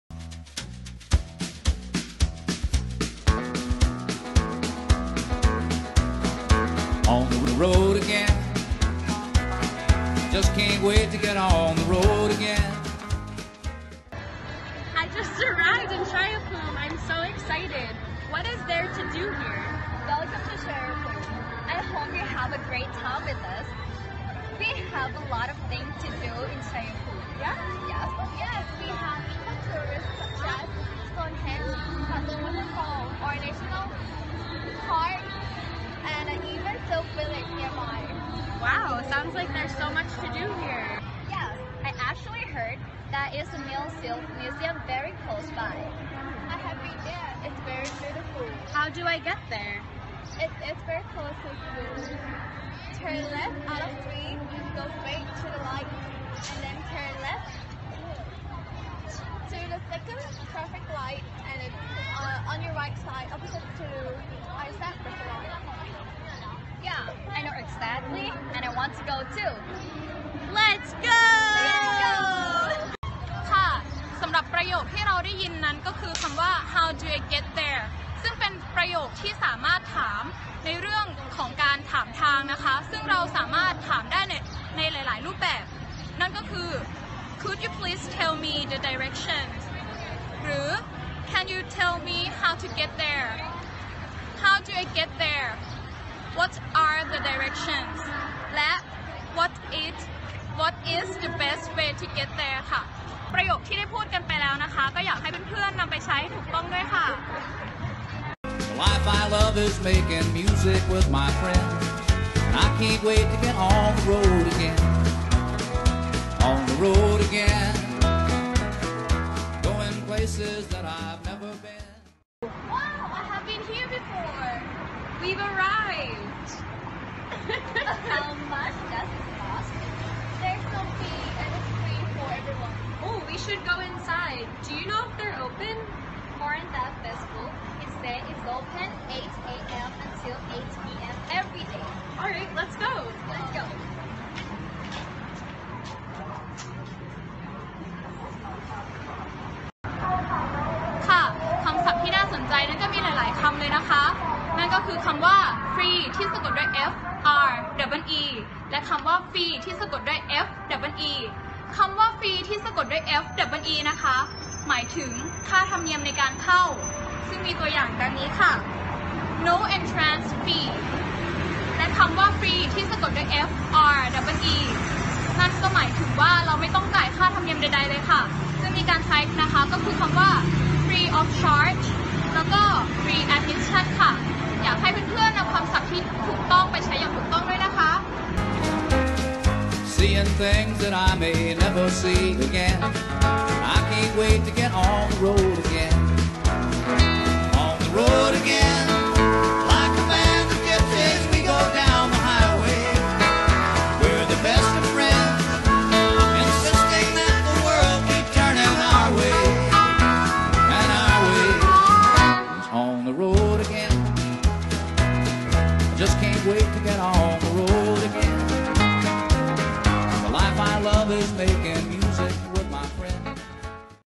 เรียนอังกฤษกับครูหนิงจิงกะเบล How do I get there EP1
How do I get there?
มาเรียนรู้ประโยคง่ายๆที่เอาไว้ใช้สอบถามเส้นทางกันนะคะ และครูหนิงยังจะพาไปเยี่ยมชมพิพิธภัณฑ์ผ้าโบราณ KOMGRISH ที่กลายเป็น landmark แห่งใหม่ของจังหวัดชัยภูมิด้วยคะ
เรียนอังกฤษกับครูหนิงจิงกะเบล